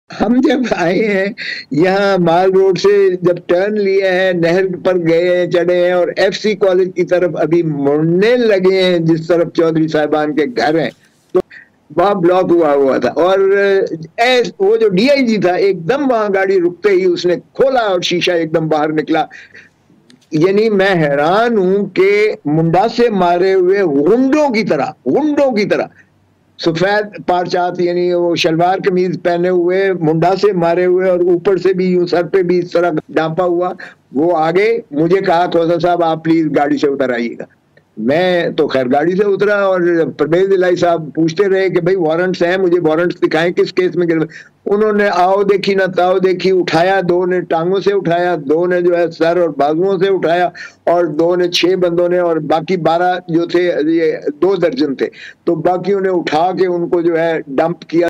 Latif Khosa Reveals Whole Incident of Pervaiz Elahi Arrest | Dawn News
Latif Khosa Nay Pervaiz Elahi Ko Giraftar Karnay Walo Ka Hulya Bata Diya | Dawn News

لطیف کھوسہ نے پرویز الہیٰ کو گرفتار کرنے والوں کا حلیہ بتادیا